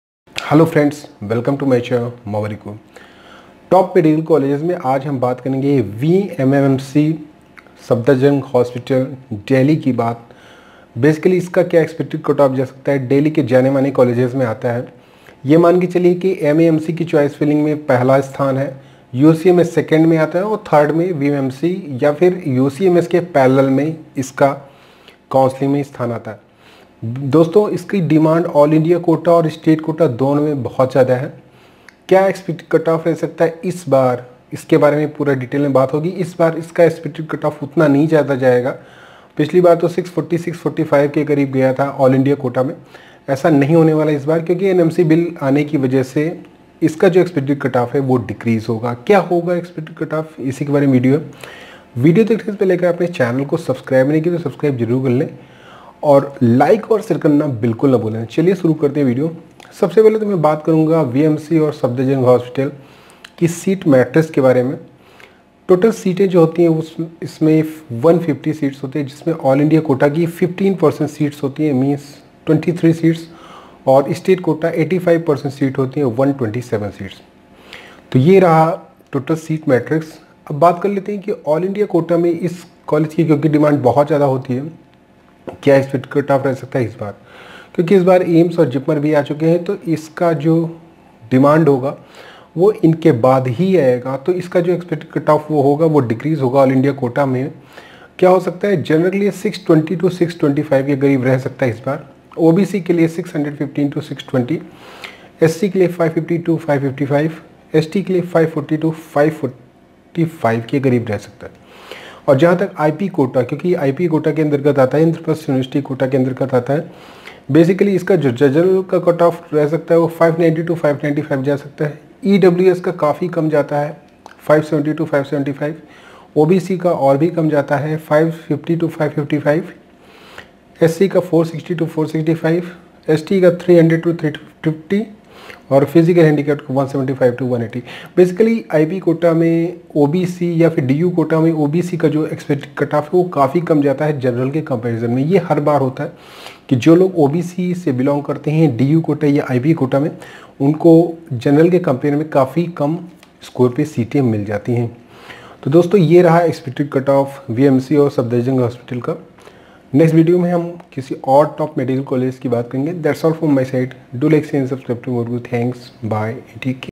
Vardhman Mahavir Medical College(VMMC.DELHI):-कितने स्कोर पर मिलेगी पक्की सीटें Category Wise !
Hello Friends,

This video is about the expected cut off for Top medical colleges Expected Cut Off For NEET UG 2020 . In this video i will talk about the  VMMC,Delhi. VMMC Delhi  is one of the top medical colleges in Delhi.

     Thanks
Maverick Guru